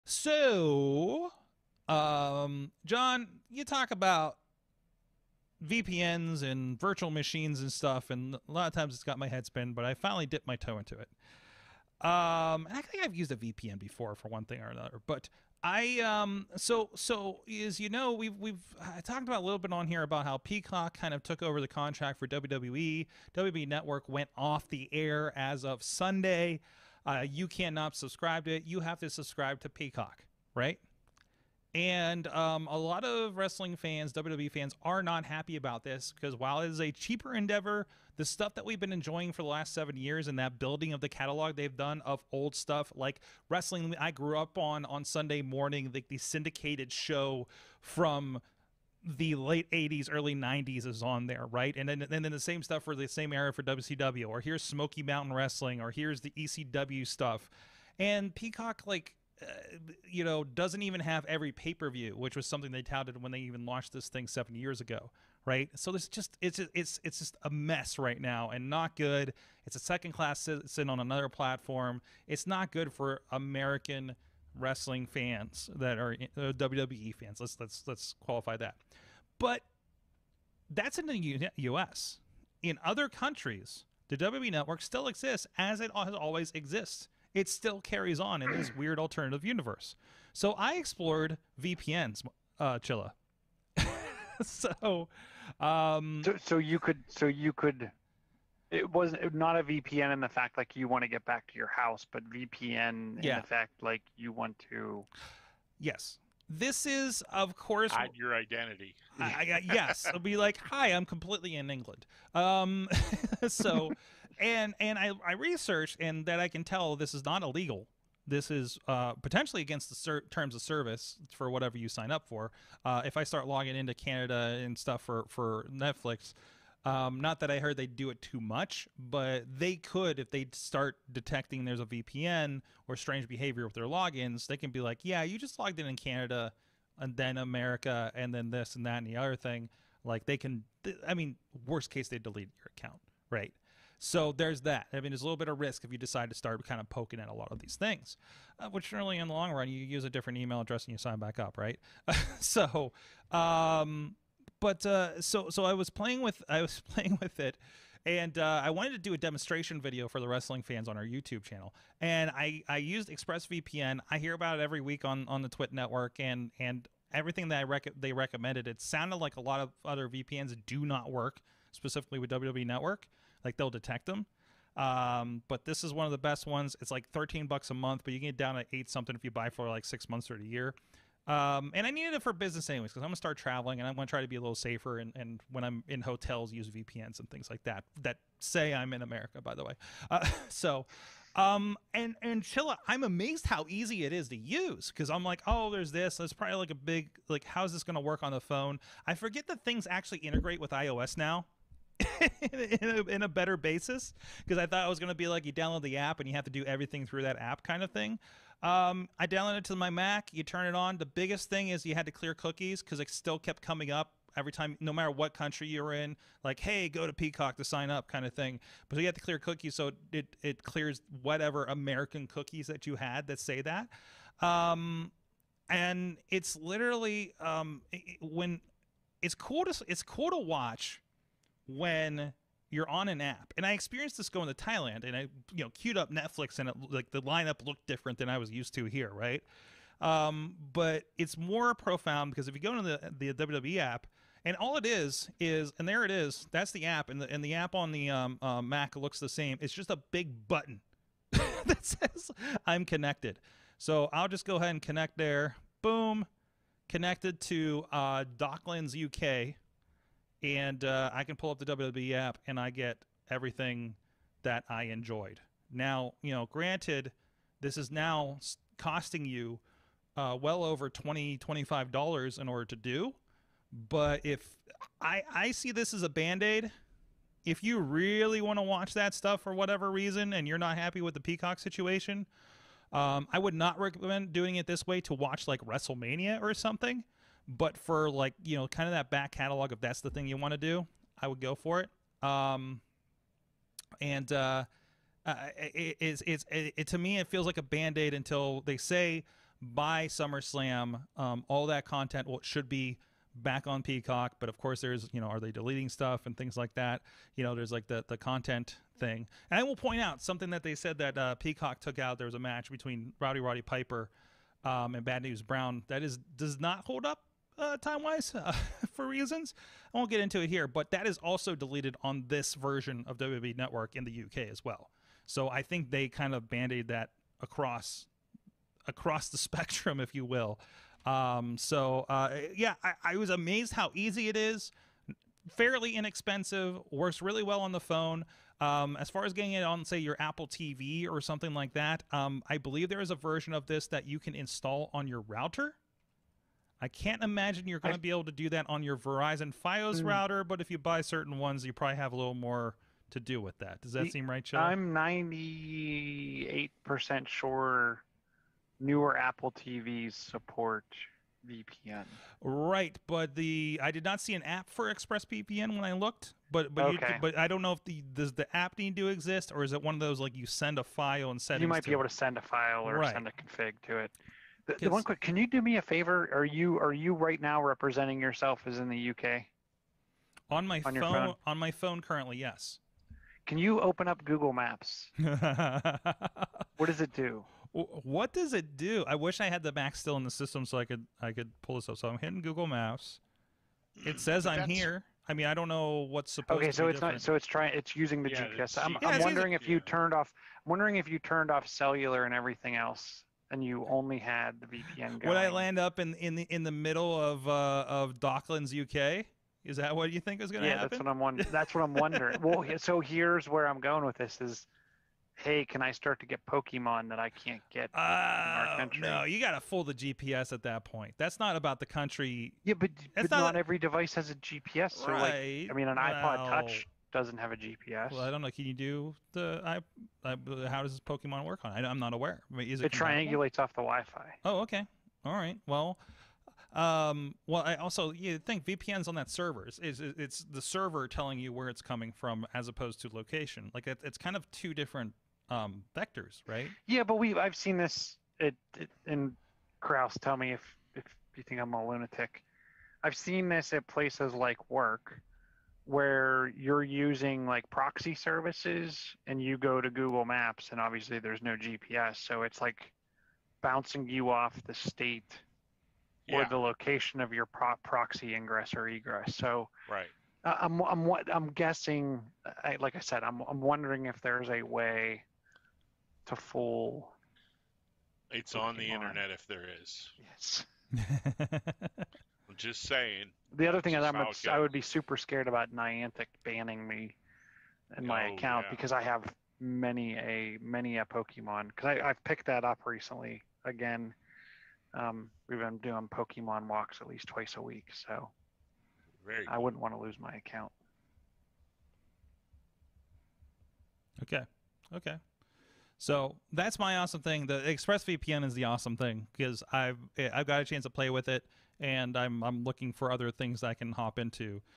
Awesome Pick: ExpressVPN for Security and More
Sorg discovered the ease of use with ExpressVPN to watch WWE Network and Netflix in other countries.

How to watch WWE Network instead of Peacock in the US with VPN